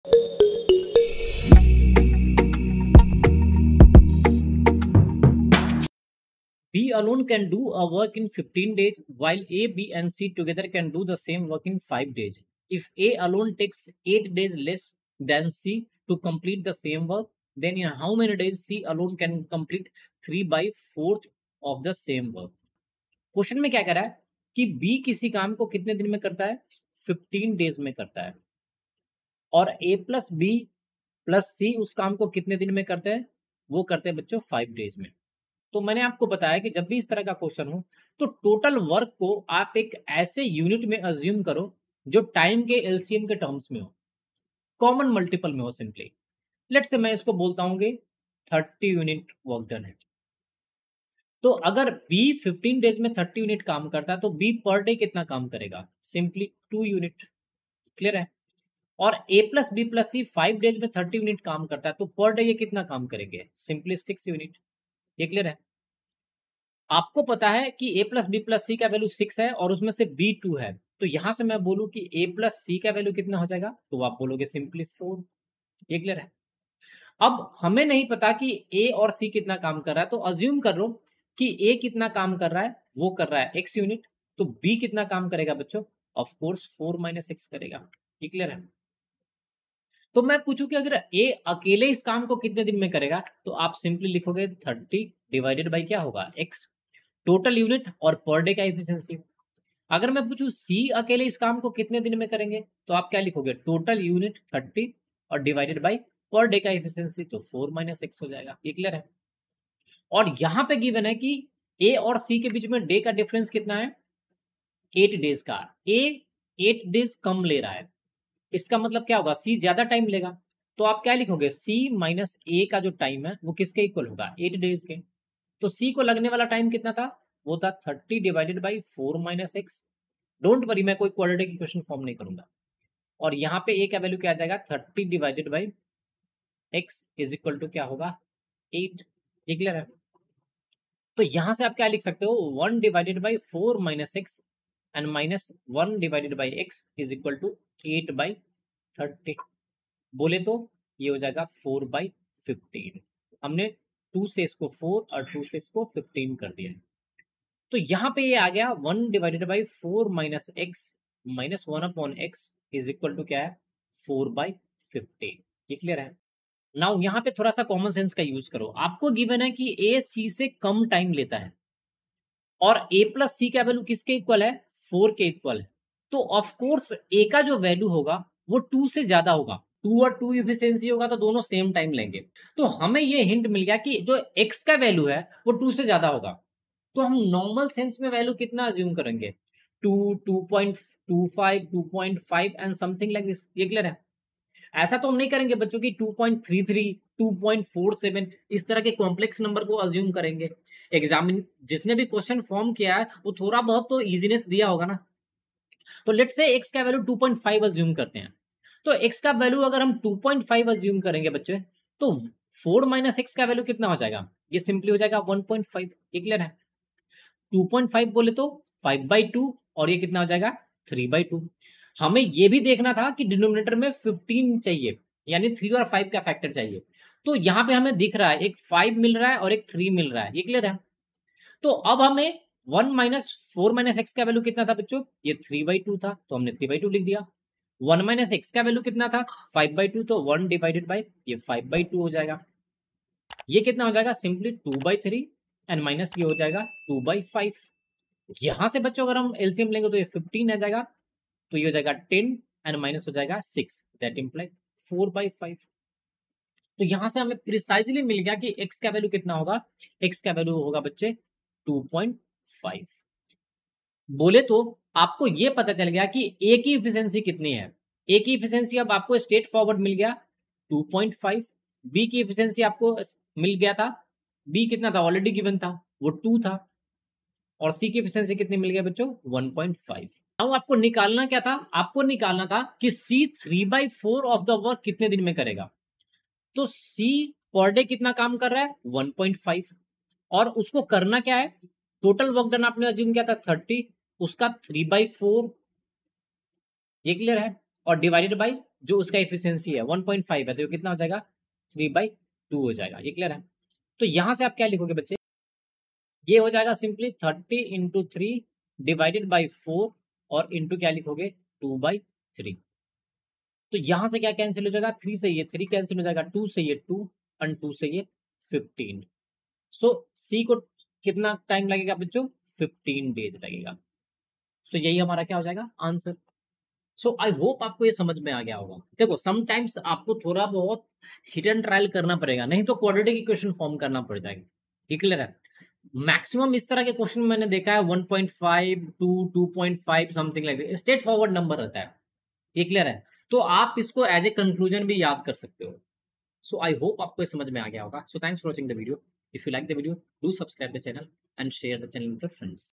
𝑩 alone can do a work in 15 days while A B & C together can do the same
𝑩 alone can do a work in 15 days, while A, B & C together can do the same work in 5 days. If 𝑨 alone takes 8 days less than C to complete the same work, then in how many days 𝑪 can complete 𝟑/𝟒 th of same work?
𝐁 अकेले एक काम को 15 दिनों में कर सकता है, जबकि 𝐀,𝐁 और 𝐂 मिलकर उसी काम को 5 दिनों में कर सकते हैं। यदि 𝐀 अकेले उसी कार्य को पूरा करने में C से 8 दिन कम लेता है, तो 𝑪 उसी कार्य का 𝟑/𝟒 भाग कितने दिनों में पूरा कर सकता है?
send your doubts at WhatsApp.